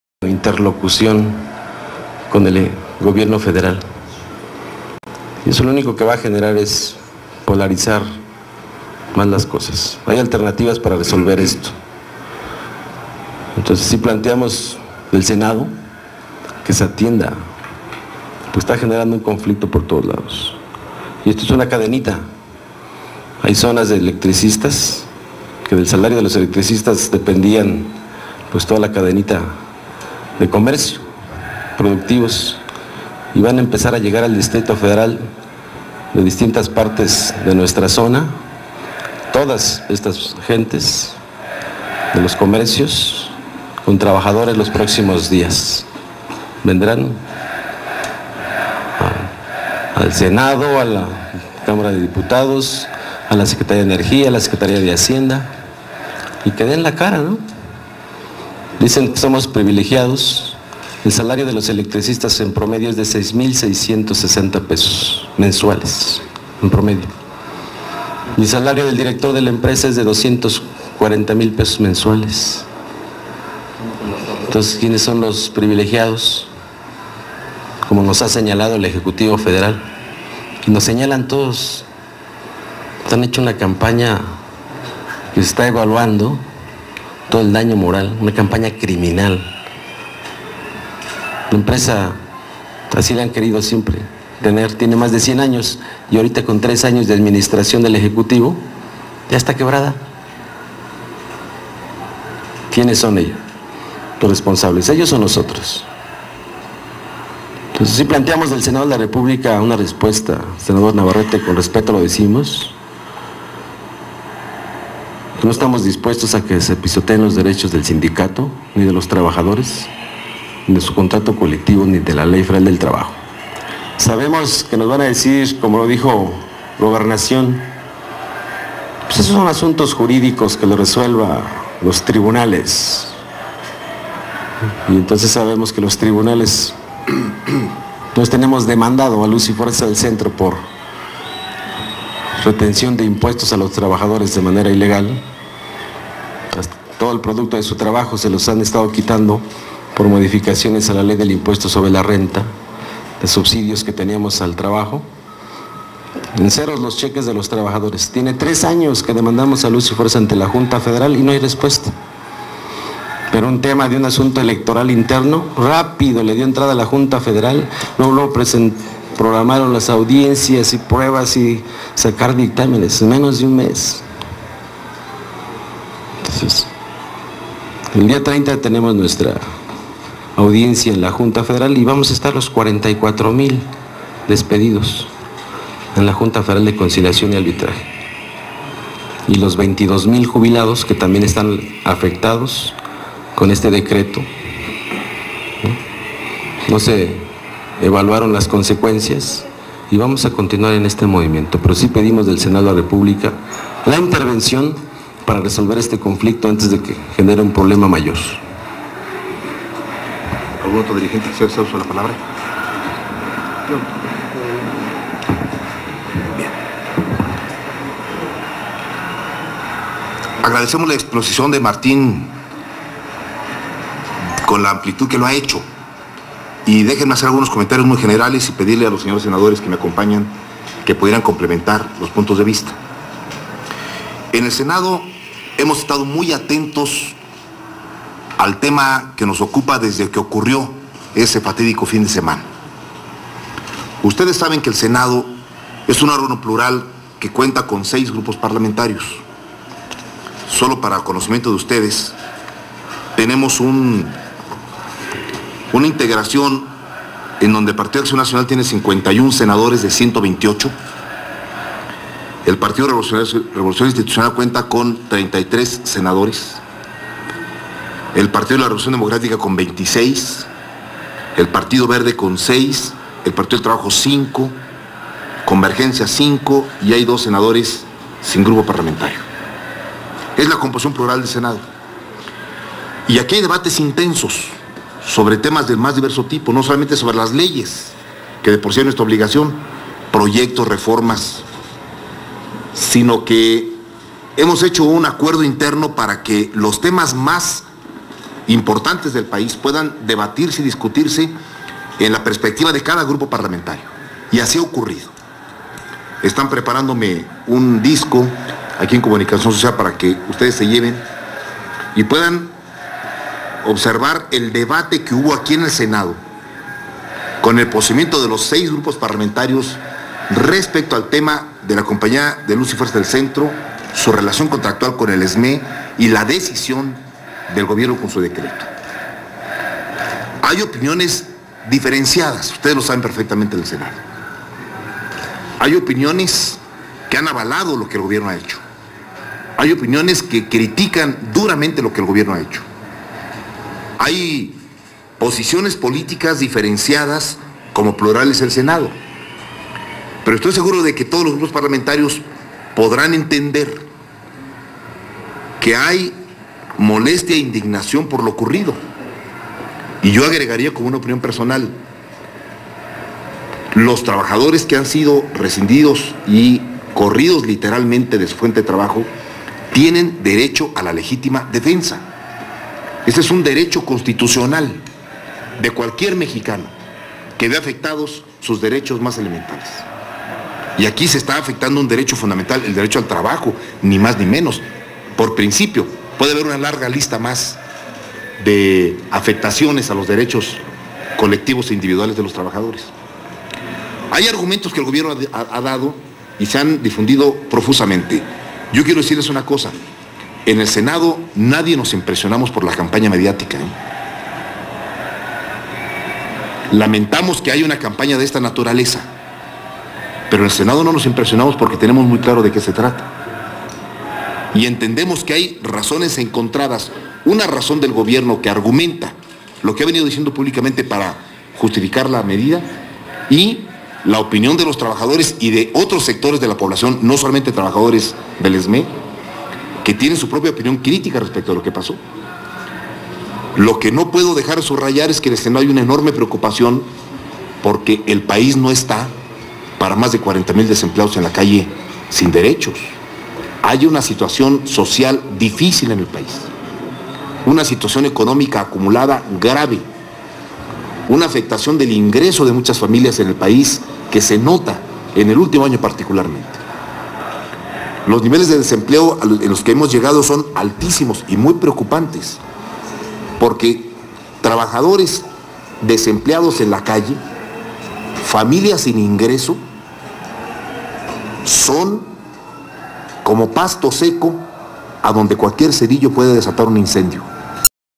Martín Esparza en el Senado
Martín Esparza habla con la Mesa Directiva del Senado. Navarrete, distinguido chucho demagógicamente da evasivas. Si ya traicionó al movimiento AMLOISTA, por que no lo haría con el SME